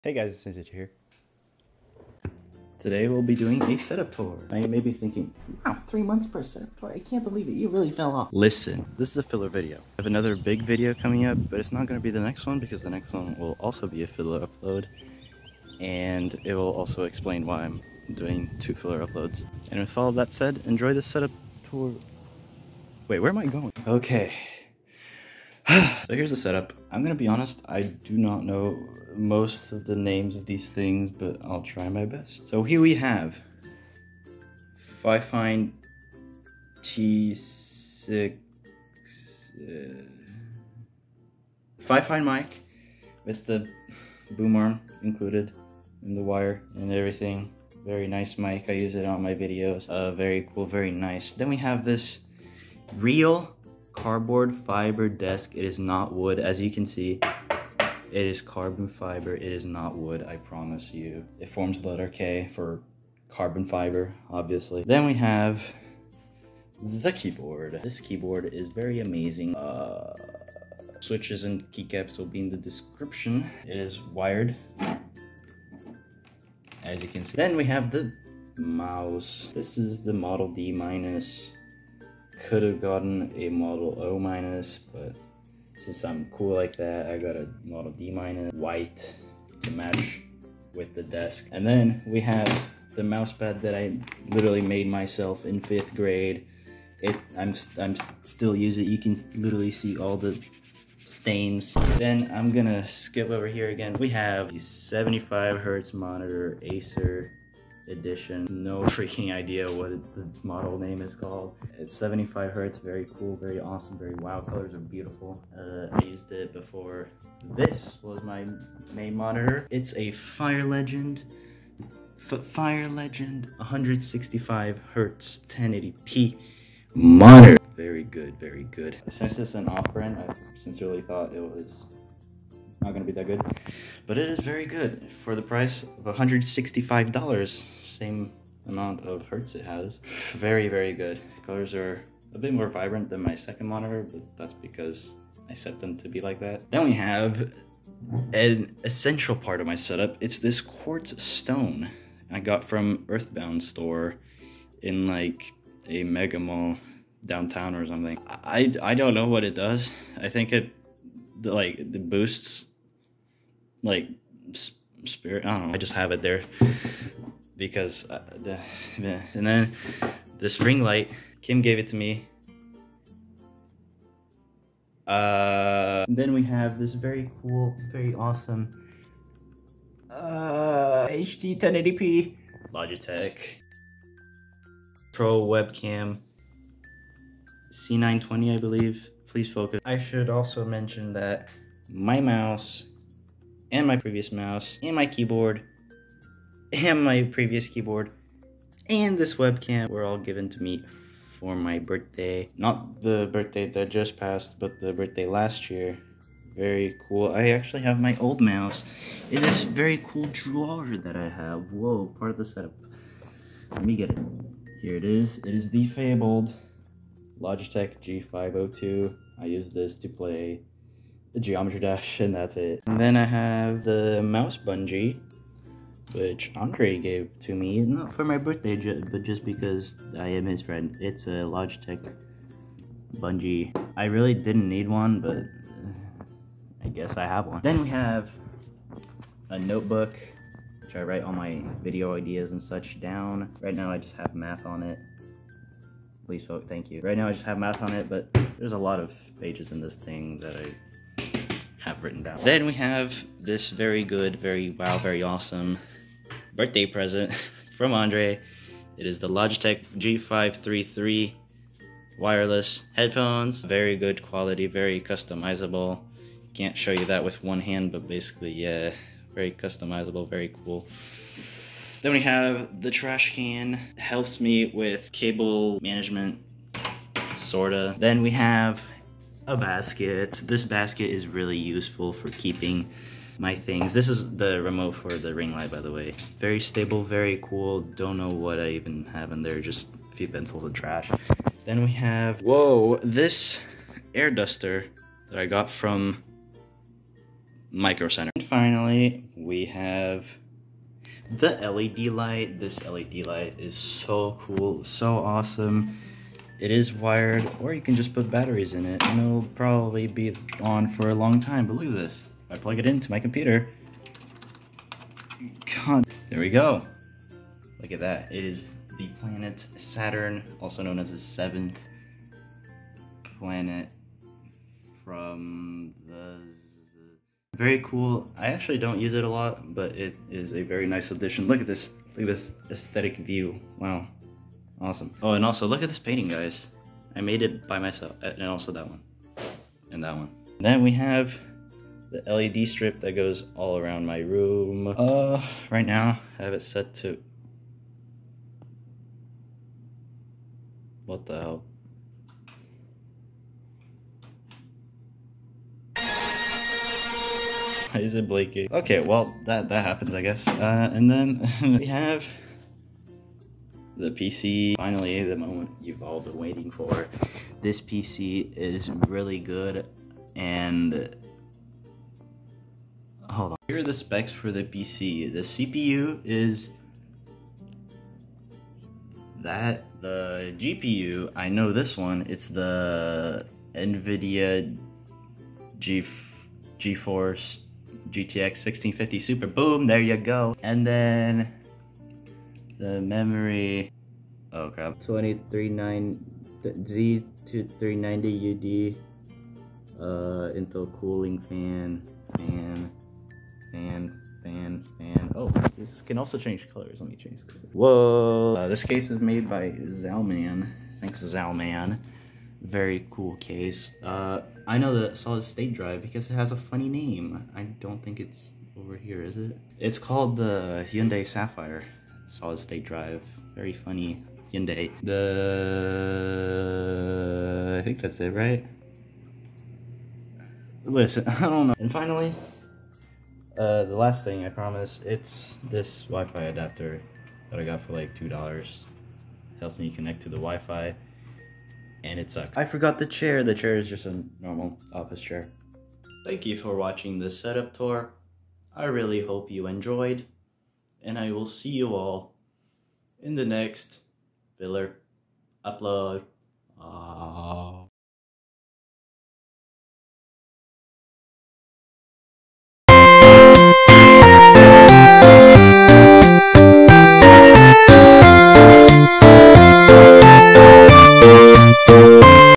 SETUP TOUR END OF 2022! (Filler Upload)
Hey guys!
This is a filler video, so don't expect much effort put into it, but expect it to be somewhat entertaining!
In this video I will show you all of the products in my beloved setup!

Now here are the links..
(links shortened with bit.ly)

PC:
dude im too lazy to look everything up

New CPU: idk yet
New webcam: i also dk yet

Softwares used:
Phone Camera App (Recording)
OBS (Recording)
Filmora 11 (Editing)

Alright that's it, thank you all for coming :D